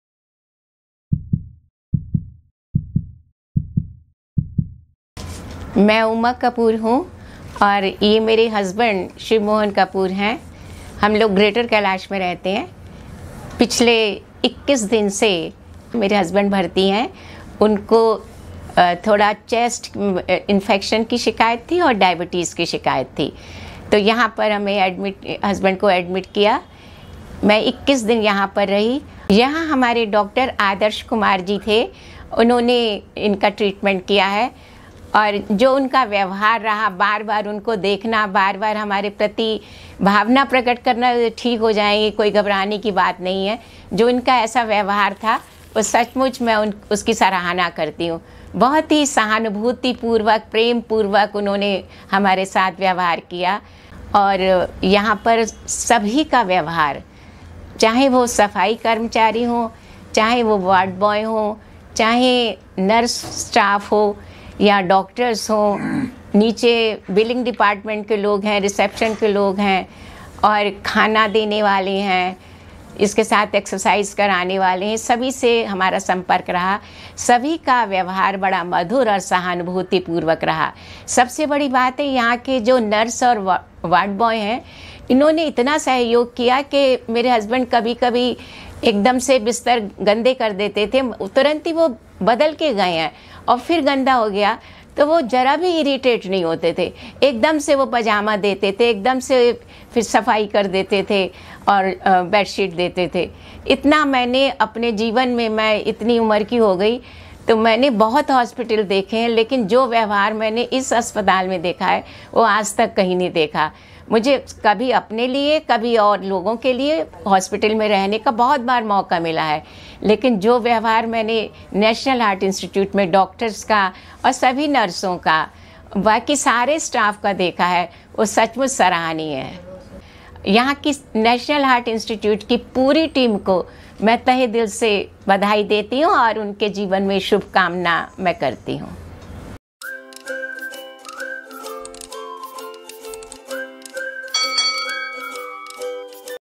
Patient Testimonial | Mr.S.M.Kapoor |National Heart Institute |Multispecialty Hospital |Dr. Adarsh
Mr. Shiv Mohan Kapoor 81 years old male presented to NHI with complaints of giddiness for 2 days, raised blood sugar (453mg/dl), and weakness. My name is Uma Kapoor and I wish to thank the doctors, nurses, and other medical professionals and staff who collectively made my experience amazing at National Heart Institute. Dr. Adarsh Kumar was thorough, informative, kind, and compassionate, and I was very confident in the course of treatment. The pleasant behaviour and positive attitude of all the doctors, nurses, paramedical and other staff were remarkable, my best wishes to all of you.